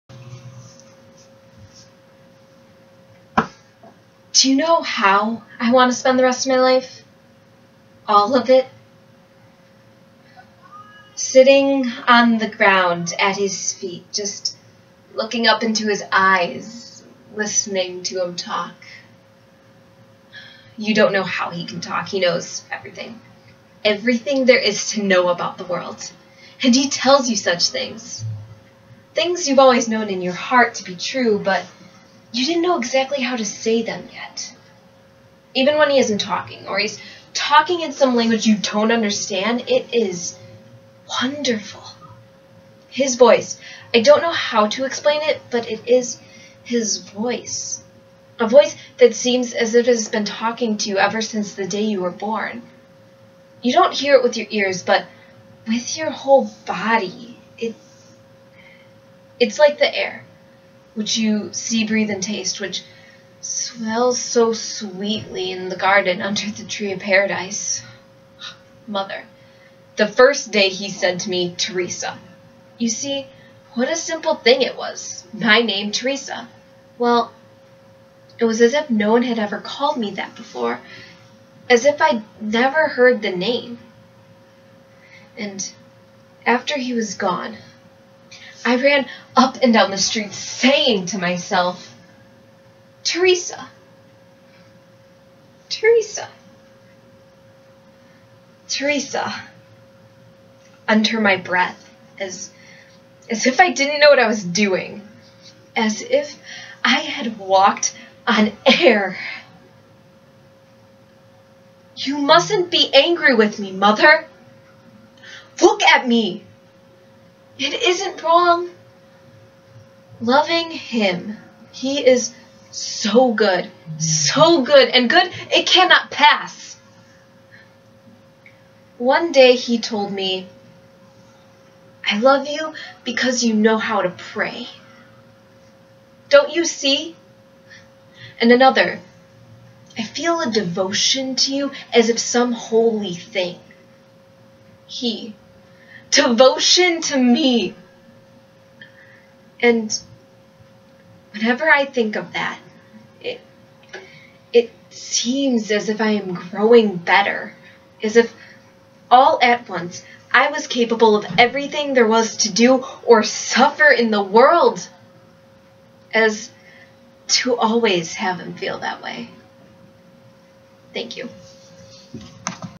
The Cradel Song by Gregrio Martinez Sierra
This my rendition of Teresa's monologue from the play  The Cradle Song by Gregorio Martinez Sierra.